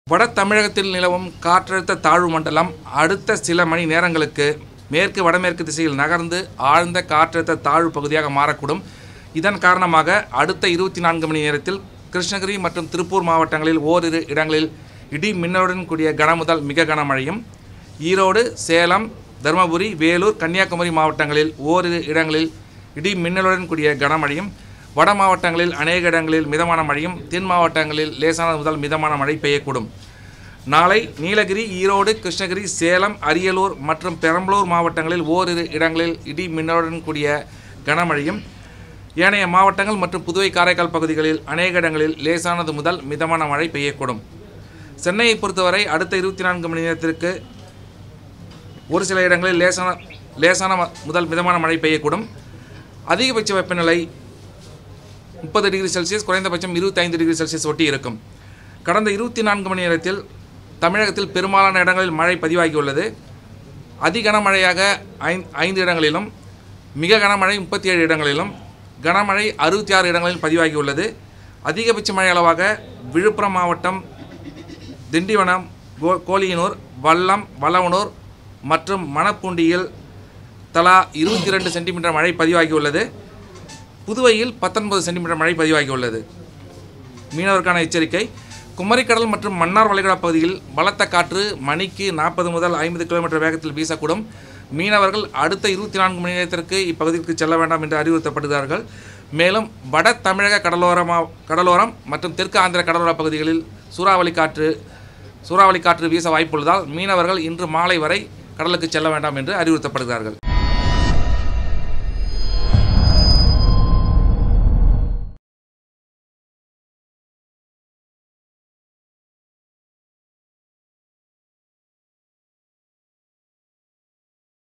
55 கிமீ வேகத்தில் வீசும் சூறாவளி ஆபத்து | Tamilnadu Weather Report | Chennai Rain | ttn
55 கிமீ வேகத்தில் வீசும் சூறாவளி ஆபத்து | Tamilnadu Weather Report | Chennai Rain | ttn